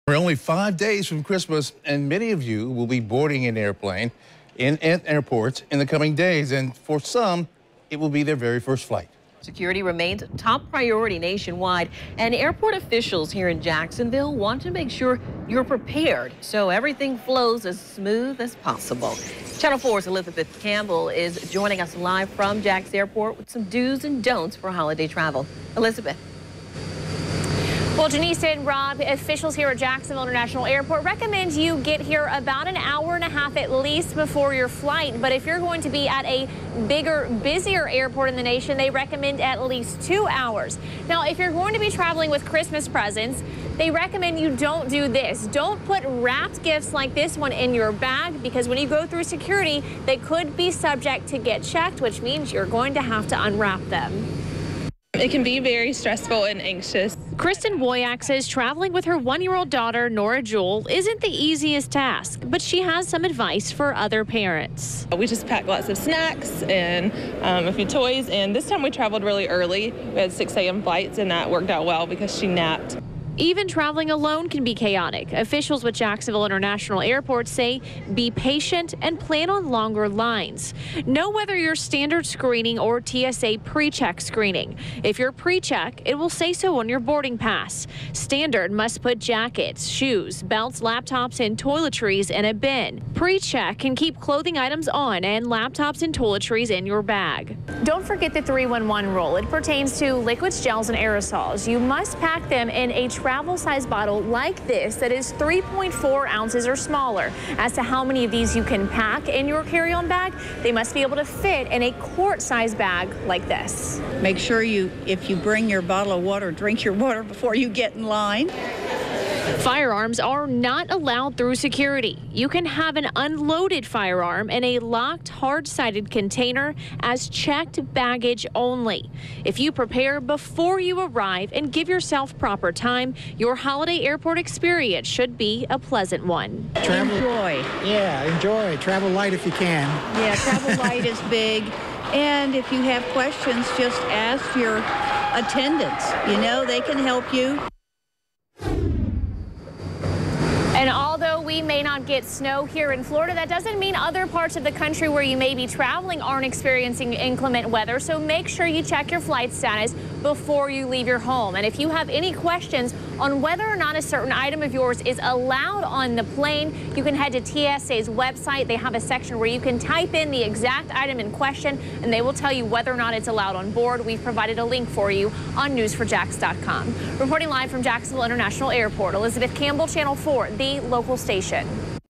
Smoothing your holiday travels
Channel 4's Elizabeth Campbell with some dos and don'ts for holiday travel.